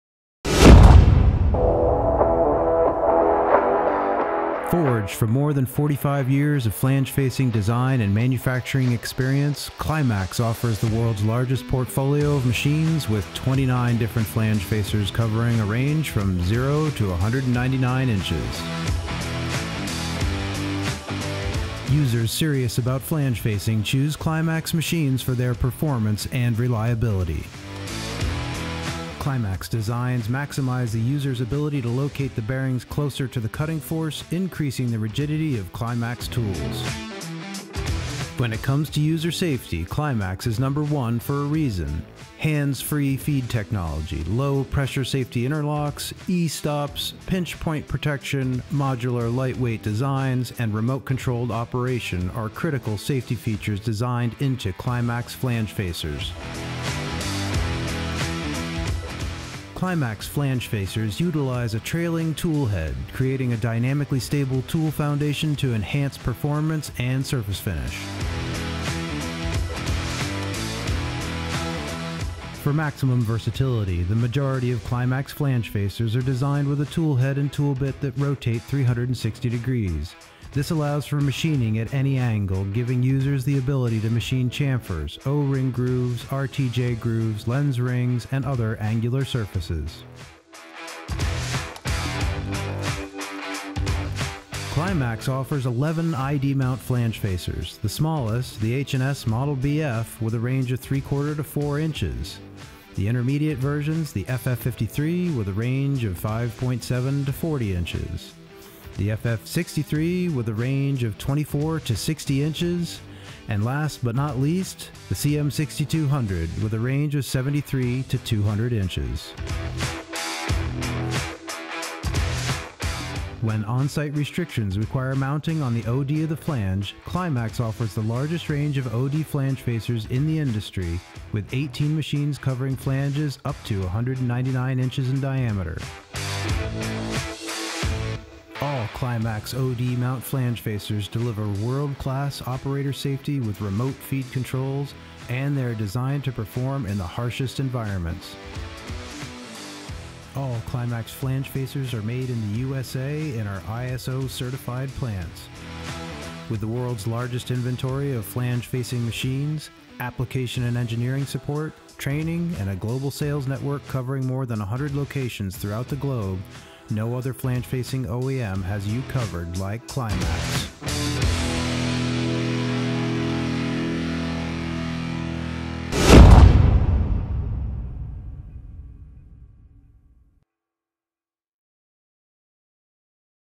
29 Flange Facers to Choose From, CLIMAX and H&S TOOL
Forged from 45+ years of flange facing experience, CLIMAX Flange Facer set the global standard for onsite flange facing. Safe, robust, and easy to set-up and run, CLIMAX flange facers provide users with the industry’s most versatile designs. Several of our machines can be configured for facing, milling, or both, and many of our models reface, mill, bevel, square, counterbore, or cut end-prep configurations for welding. With 29 models to choose from, CLIMAX flange facers can tackle an unrivaled range of applications from 0 to 199 inches (0 to 5054 mm).